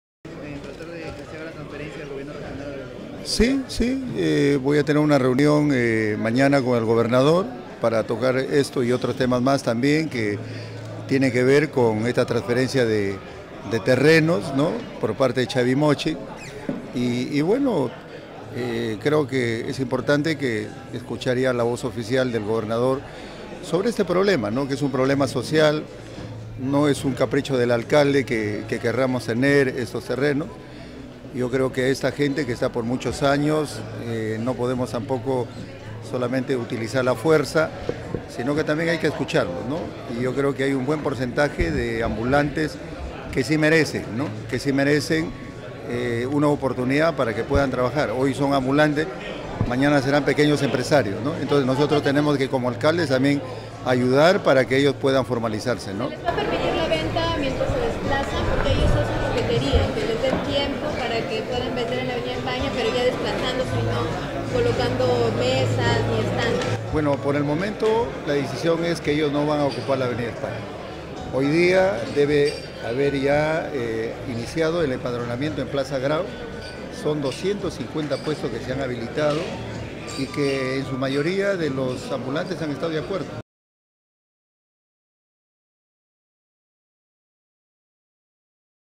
Comercio informal: Marcelo no pierde la fe
El alcalde de Trujillo, Daniel Marcelo, no pierde la esperanza de lograr la donación de un terreno por parte del Gobierno Regional de La Libertad para la reubicación de los vendedores ambulantes.